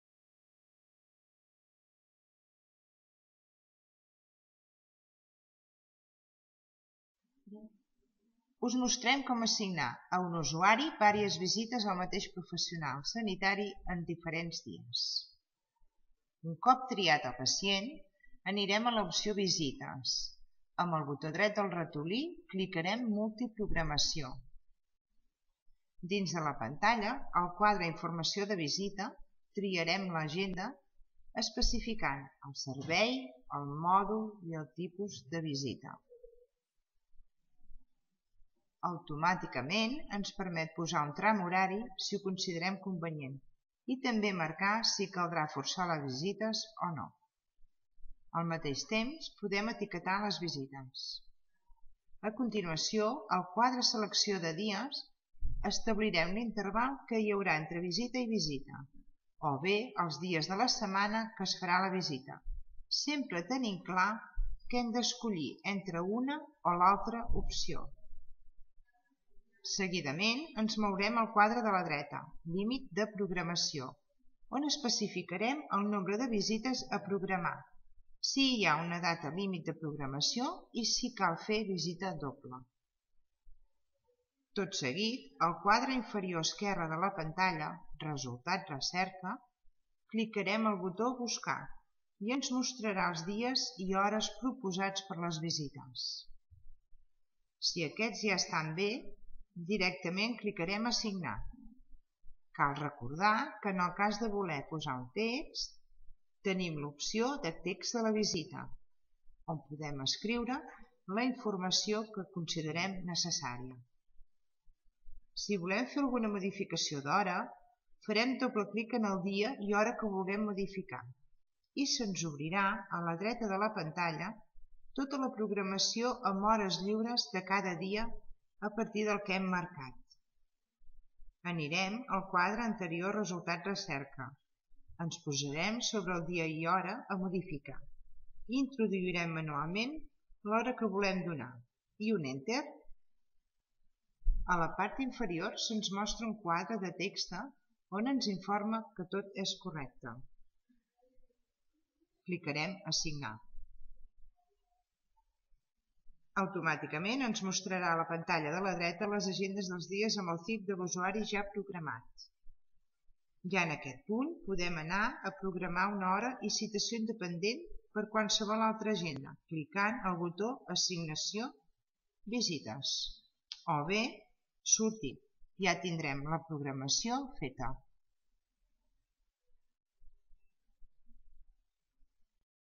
Anem per feina; la multiprogramació de visites a l'ECAP
En aquest vídeo tutorial us ensenyem com donar diferents hores de visita per a una mateixa agenda.